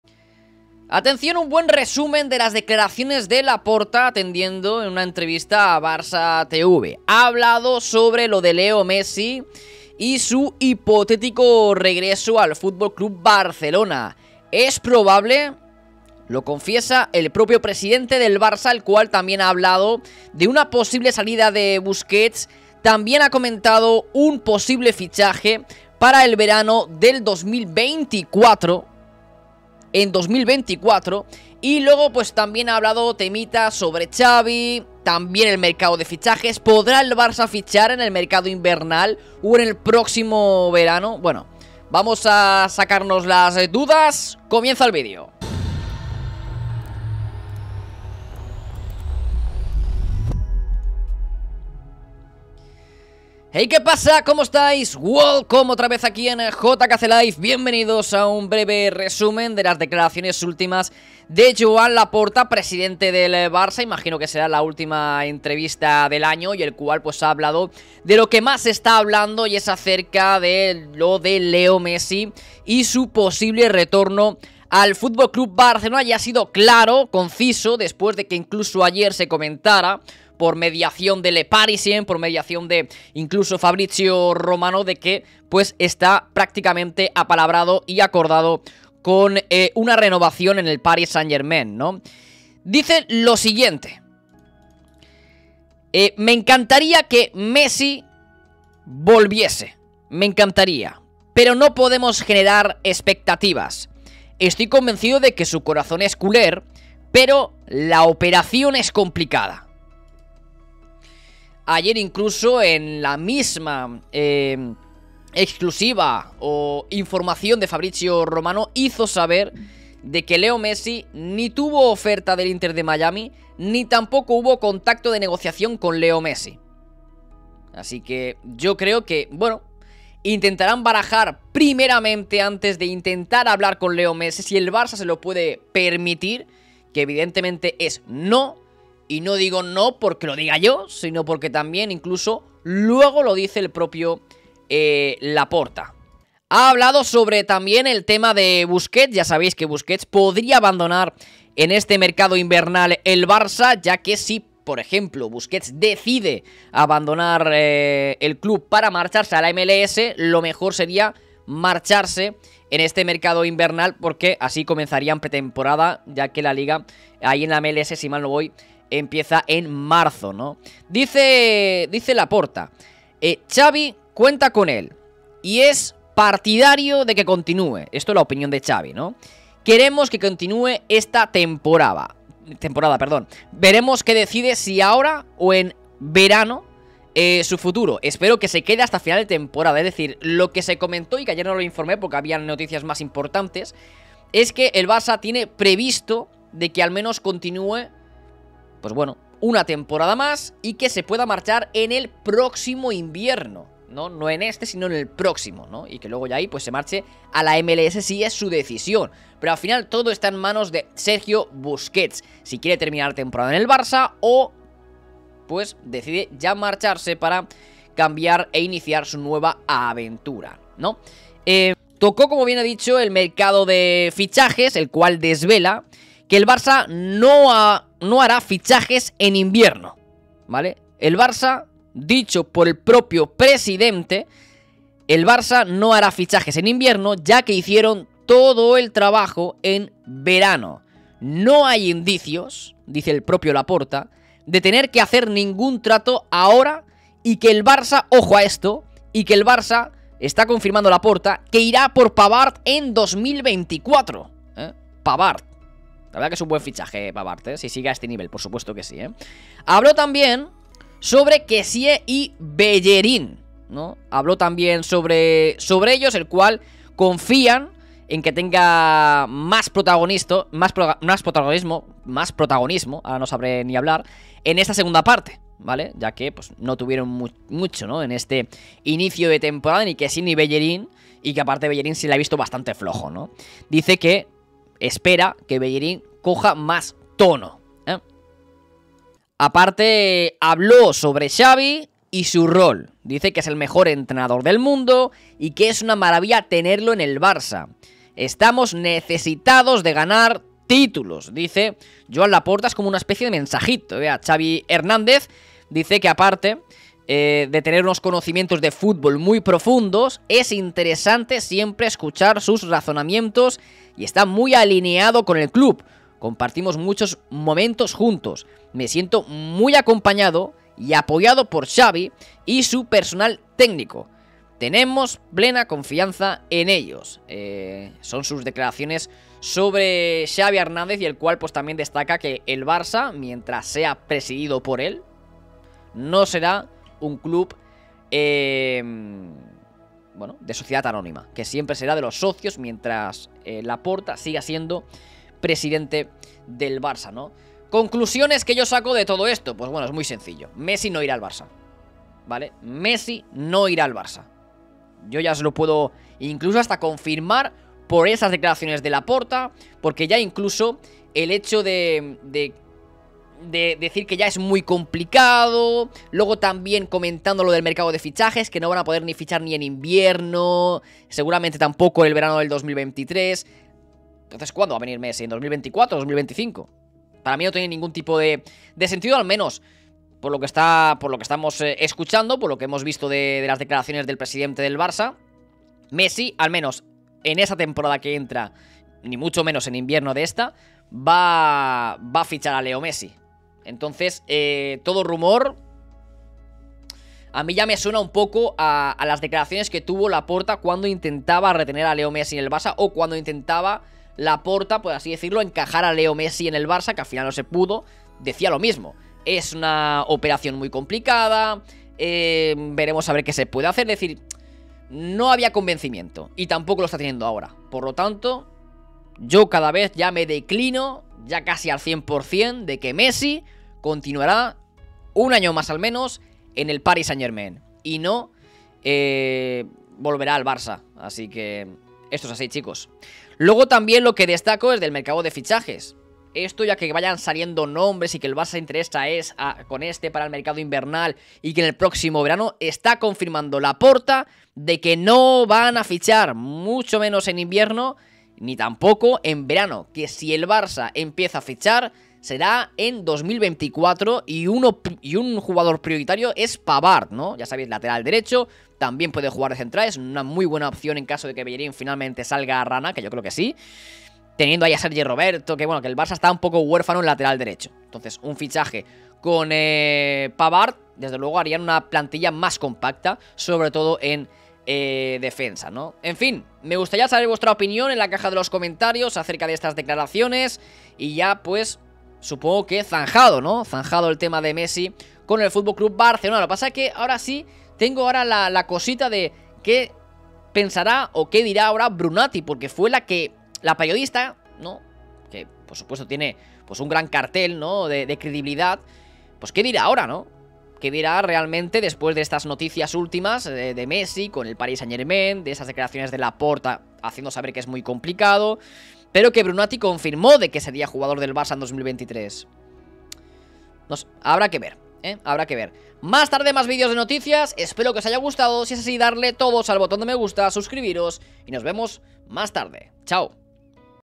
🚨¿MESSI al BARÇA? LAPORTA DEJA CLARO las POSIBILIDADES y HABLA de FICHAJES. OBJETIVO PAVARD en 2024
Laporta atendiendo en BarçaTV y habló si hay posibilidades de que Messi fiche por el Barça.
Habló de la salida de Busquets, también sobre Kessié y Bellerín. Comenta el mercado de fichajes. ¿Podrá fichar el Barça?
También halagó a Xavi. "Es el mejor entrenador del mundo".